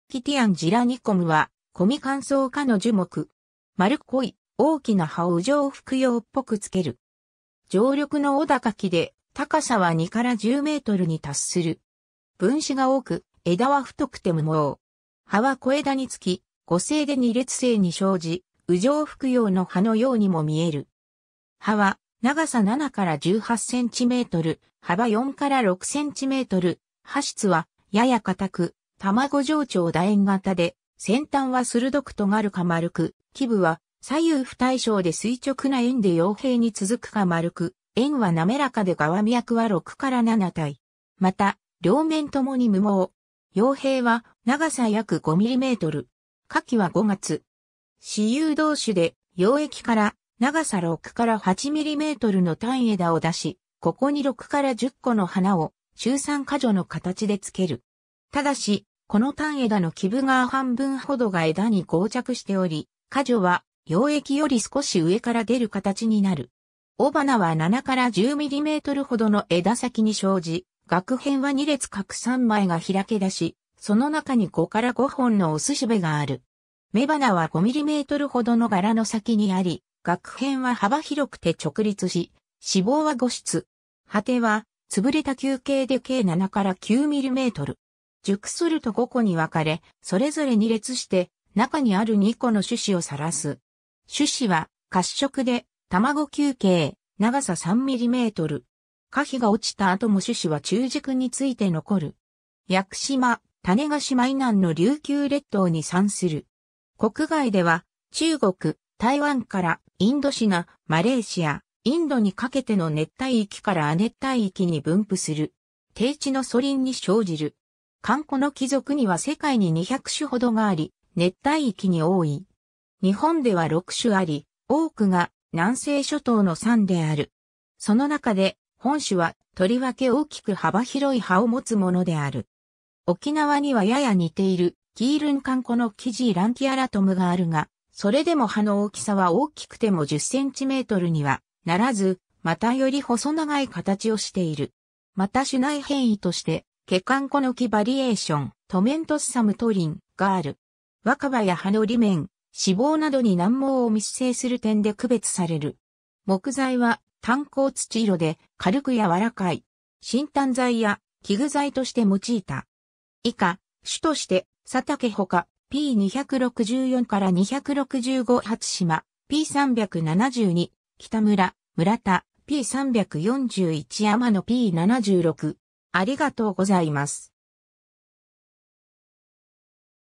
カキバカンコノキ
カキバカンコノキ Glochidion zeylanicum (Gaertn.) はコミカンソウ科（トウダイグサ科）の樹木。
丸っこい大きな葉を羽状複葉っぽくつける。
 常緑の小高木で、高さは2-10mに達する。
分枝が多く、枝は太くて無毛。
葉は小枝につき、互生で二列生に生じ、羽状複葉の葉のようにも見える。
葉は長さ7-18cm、幅4-6cm、葉質はやや硬く、卵状長楕円形で先端は鋭く尖るか丸く、基部は左右不対称で垂直な縁で葉柄に続くか丸く、縁は滑らかで側脈は6-7対。
また、両面共に無毛。
葉柄は長さ約5mm。
 花期は5月。
雌雄同株で、葉腋から長さ6-8mmの短枝を出し、ここに6-10個の花を集散花序の形で着ける。
ただし、この短枝の基部側半分ほどが枝に合着しており、花序は葉腋より少し上から出る形になる。
雄花は7-10mmほどの枝先に生じ、萼片は二列各3枚が開出し、その中に5-5本の雄蘂がある。
雌花は5mmほどの柄の先にあり、萼片は幅広くて直立し、子房は5室。
蒴果は潰れた球形で径7-9mm。
熟すると5個に分かれ、それぞれ二裂して中にある2個の種子を晒す。
種子は褐色で卵球形、長さ3mm。
果皮が落ちた後も種子は中軸について残る。
 屋久島、種子島以南の琉球列島に産する。
国外では中国、台湾からインドシナ、マレーシア、インドにかけての熱帯域から亜熱帯域に分布する。
低地の疎林に生じる。
 カンコノキ属には世界に200種ほどがあり、熱帯域に多い。
日本では6種あり、多くが南西諸島の産である。
その中で本種はとりわけ大きく幅広い葉を持つものである。
沖縄にはやや似ているキールンカンコノキ G. lanceolatum があるが、それでも葉の大きさは大きくても10cmにはならず、またより細長い形をしている。
 また種内変異として、ケカンコノキ var. tomentosum Trin. がある。
若葉や葉の裏面、子房などに軟毛を密生する点で区別される。
 木材は淡黄土色で、軽く柔らかい。
薪炭材や器具材として用いた。
 ^ 以下、主として佐竹他(1981),p.264-265 ^ 初島(1975),p.372 ^ 北村・村田(1971),p.341 ^ 天野(1982)p.76